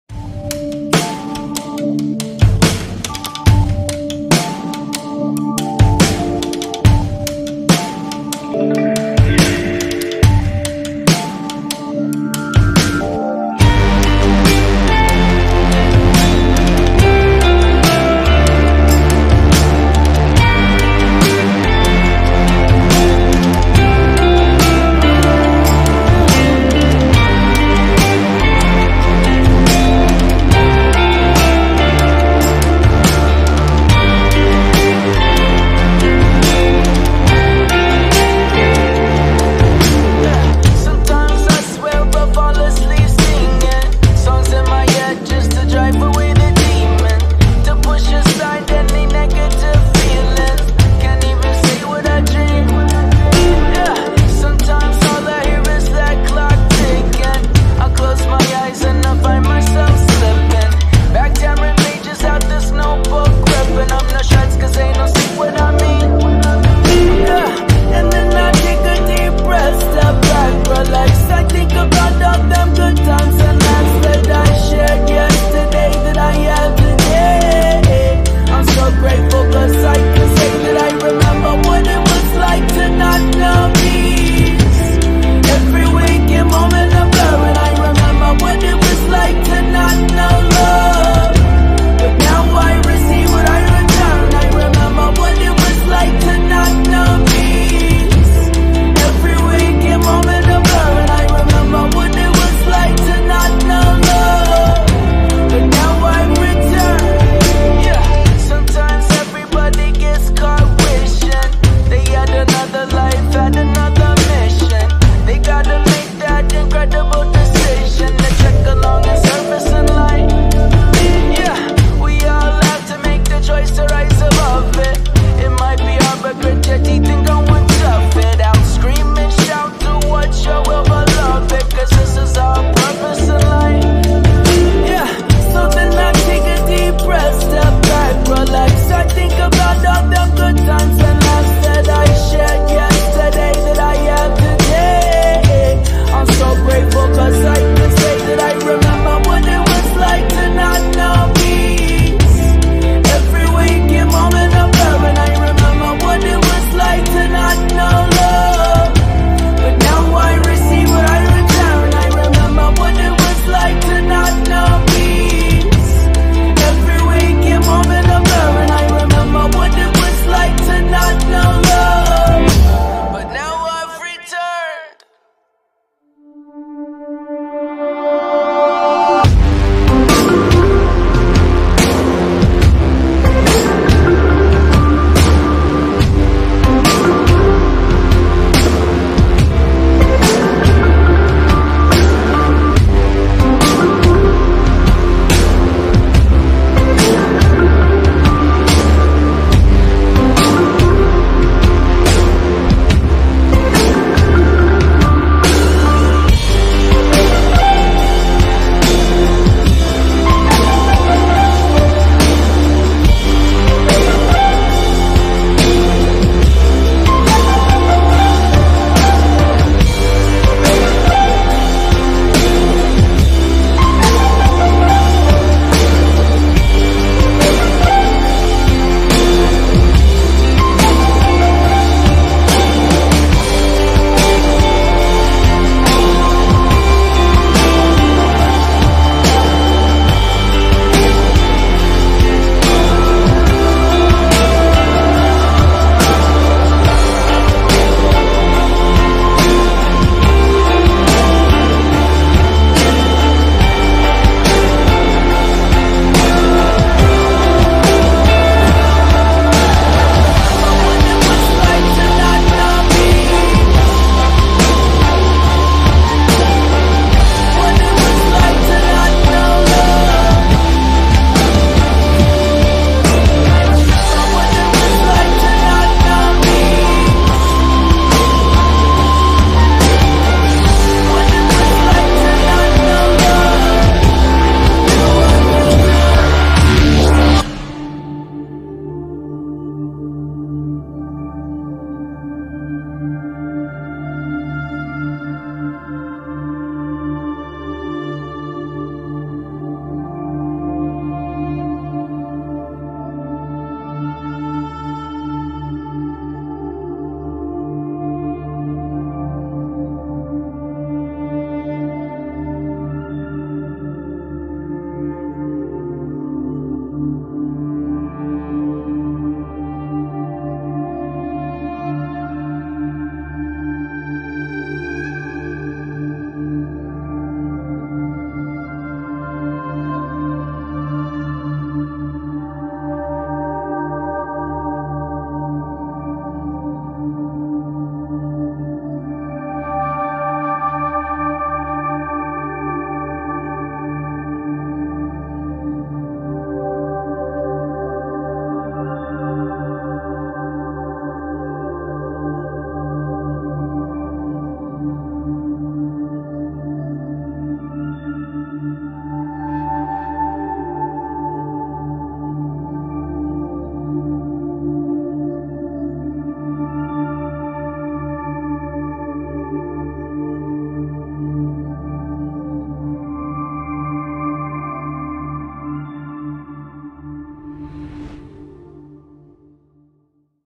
Открытие водного сезона '20 (flysurfer soul 8)
Видео просто собрано из того что оказалось на гопро) оно автосгенерированное